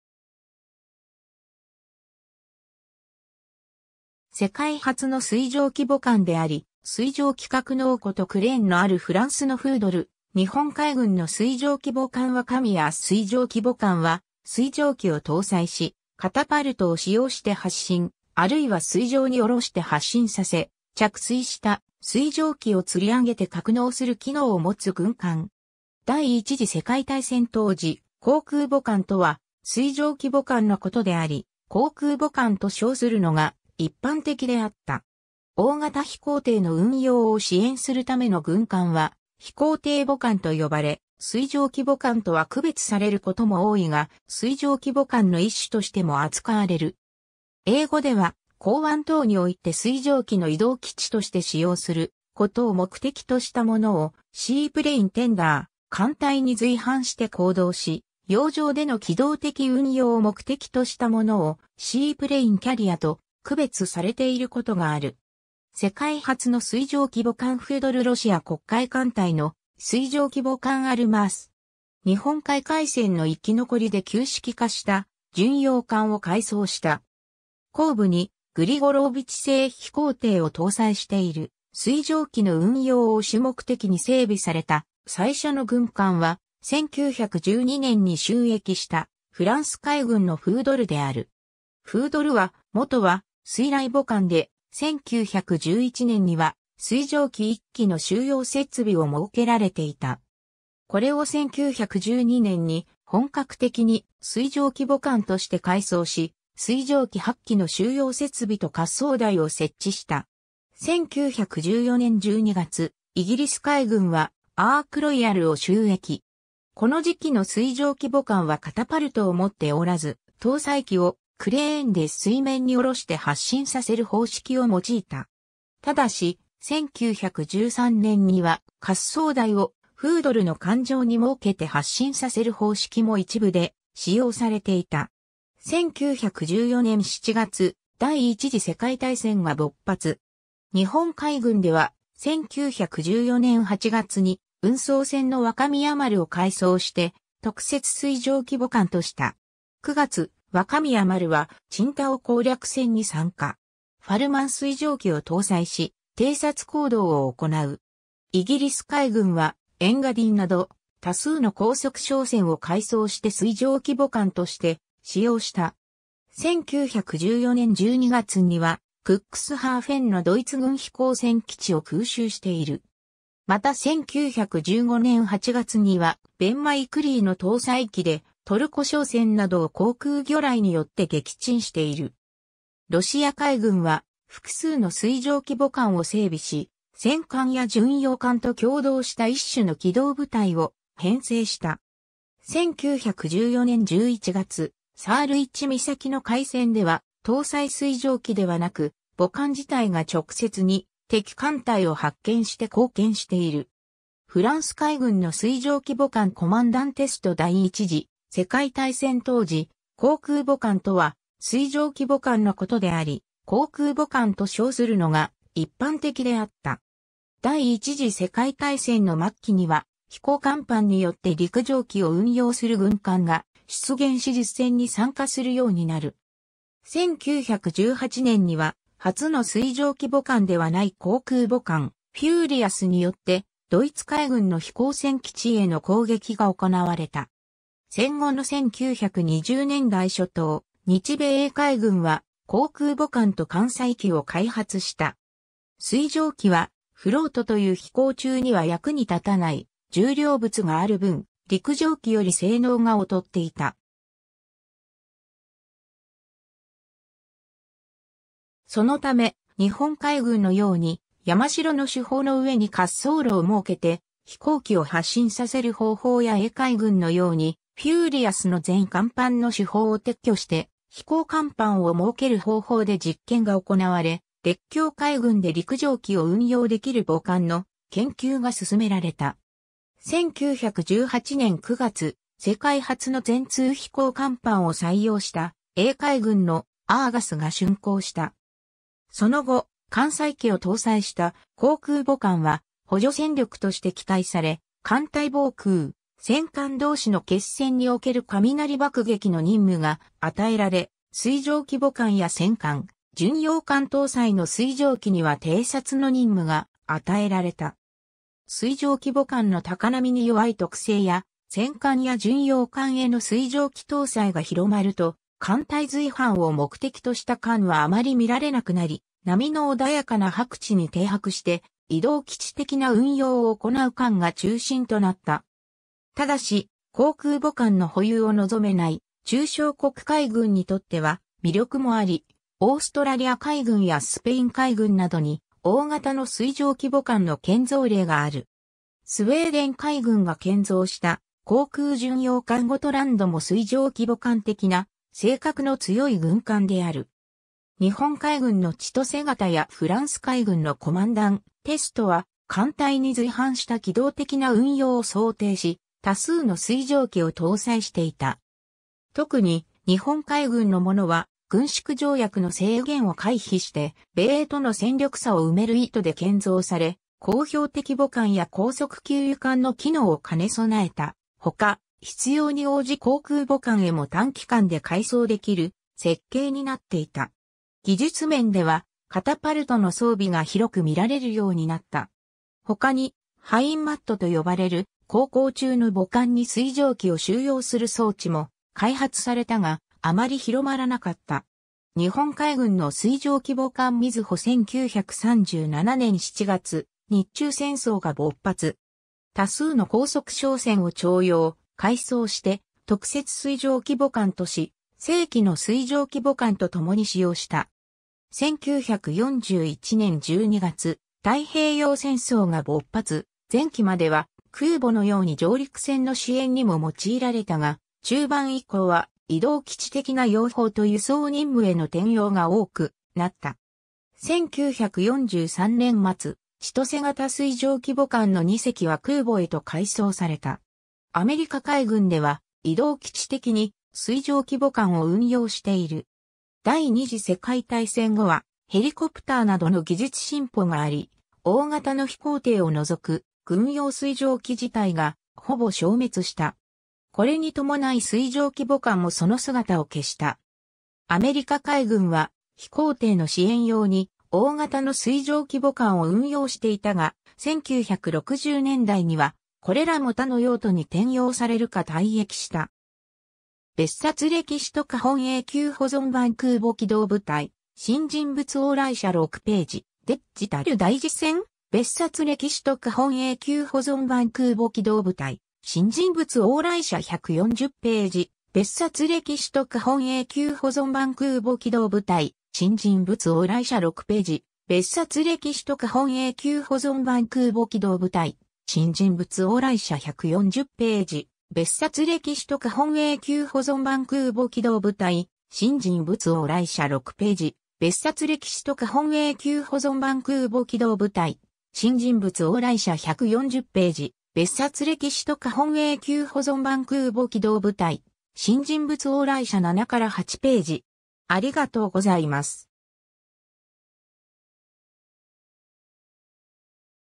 水上機母艦
世界初の水上機母艦であり、水上機格納庫とクレーンのあるフランスの フードル (1912年) 日本海軍の水上機母艦若宮 水上機母艦（すいじょうきぼかん）は、水上機を搭載し、カタパルトを使用して発進、あるいは水上に降ろして発進させ、着水した水上機を吊り上げて格納する機能を持つ軍艦。
第一次世界大戦当時、「航空母艦」とは水上機母艦のことであり、「航空母艦」と称するのが一般的であった。
 大型飛行艇の運用を支援するための軍艦は飛行艇母艦と呼ばれ、水上機母艦とは区別されることも多いが、水上機母艦の一種としても扱われる。
 英語では、港湾等において水上機の移動基地として使用することを目的としたものをSeaplane tender、艦隊に随伴して行動し、洋上での機動的運用を目的としたものをSeaplane carrierと区別されていることがある。
 世界初の水上機母艦フードル ロシア黒海艦隊の水上機母艦アルマース。
日本海海戦の生き残りで旧式化した巡洋艦を改装した。
後部にグリゴローヴィチ製飛行艇を搭載している 水上機の運用を主目的に整備された最初の軍艦は、1912年に就役したフランス海軍のフードルである。
フードルは元は水雷母艦で、1911年には水上機1機の収容設備を設けられていた。
これを1912年に本格的に水上機母艦として改装し、水上機8機の収容設備と滑走台を設置した。
1914年12月、イギリス海軍はアーク・ロイヤルを就役。
 この時期の水上機母艦はカタパルトを持っておらず、搭載機をクレーンで水面に下ろして発進させる方式を用いた。
ただし、1913年には、滑走台をフードルの艦上に設けて発進させる方式も一部で使用されていた。
 1914年7月、第一次世界大戦が勃発。
日本海軍では、1914年8月に運送船の若宮丸を改装して特設水上機母艦とした。
9月、若宮丸は青島攻略戦に参加。
ファルマン水上機を搭載し、偵察行動を行う。
 イギリス海軍は、エンガディンなど多数の高速商船を改装して水上機母艦として使用した。
1914年12月にはクックスハーフェンのドイツ軍飛行船基地を空襲している（en:Raid on Cuxhaven）。
また1915年8月には、ベン・マイ・クリーの搭載機でトルコ商船などを航空魚雷によって撃沈している。
ロシア海軍は複数の水上機母艦を整備し、戦艦や巡洋艦と協同した一種の機動部隊を編成した。
1914年11月、サールィチ岬の海戦では、搭載水上機ではなく、母艦自体が直接に敵艦隊を発見して貢献している。
 フランス海軍の水上機母艦コマンダン・テスト 第一次世界大戦当時、「航空母艦」とは水上機母艦のことであり、「航空母艦」と称するのが一般的であった。
第一次世界大戦の末期には飛行甲板によって陸上機を運用する軍艦（後の航空母艦）が出現し実戦に参加するようになる。
1918年には初の水上機母艦ではない航空母艦「フューリアス」によってドイツ海軍の飛行船基地への攻撃が行われた。
戦後の1920年代初頭、日米英海軍は航空母艦と艦載機を開発した。
水上機はフロートという飛行中には役に立たない重量物がある分、陸上機より性能が劣っていた。
そのため、日本海軍のように「山城」の主砲の上に滑走路を設けて飛行機を発進させる方法や英海軍のように「フューリアス」の前甲板の主砲を撤去して飛行甲板を設ける方法で実験が行われ、列強海軍で陸上機を運用できる母艦の研究が進められた。
1918年9月、世界初の全通飛行甲板を採用した英海軍の「アーガス」が竣工した。
その後、艦載機を搭載した航空母艦は補助戦力として期待され、艦隊防空、戦艦同士の決戦における雷爆撃の任務が与えられ、水上機母艦や戦艦・巡洋艦搭載の水上機には偵察の任務が与えられた。
 水上機母艦の高波に弱い特性や戦艦や巡洋艦への水上機搭載が広まると艦隊随伴を目的とした艦はあまり見られなくなり、波の穏やかな泊地に停泊して移動基地的な運用を行う艦が中心となった。
ただし、航空母艦の保有を望めない中小国海軍にとっては魅力もあり、オーストラリア海軍やスペイン海軍などに大型の水上機母艦の建造例がある。
スウェーデン海軍が建造した航空巡洋艦ゴトランドも水上機母艦的な性格の強い軍艦である。
日本海軍の千歳型やフランス海軍のコマンダン・テストは、艦隊に随伴した機動的な運用を想定し、多数の水上機を搭載していた。
特に日本海軍のものは、軍縮条約の制限を回避して米英との戦力差を埋める意図で建造され、甲標的母艦や高速給油艦の機能を兼ね備えたほか、必要...